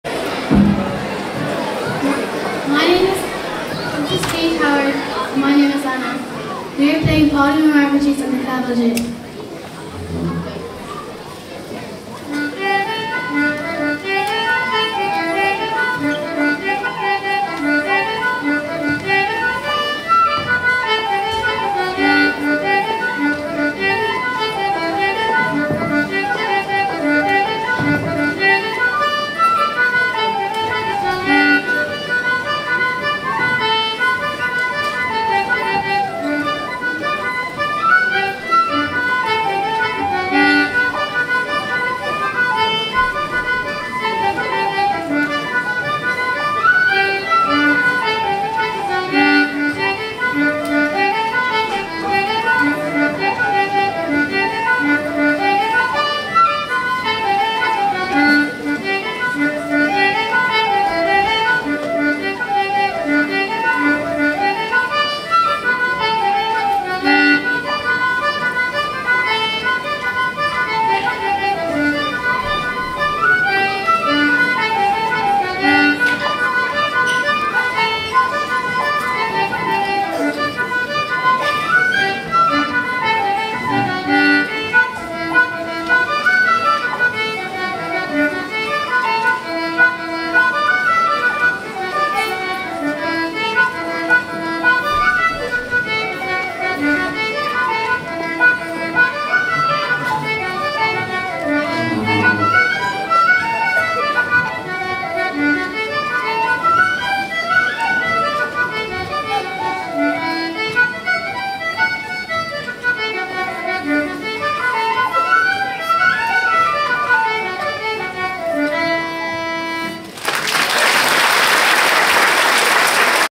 Trad Music Interlude 6
Barefield National School senior spring concert , Tuesday February 21st at Glór, Ennis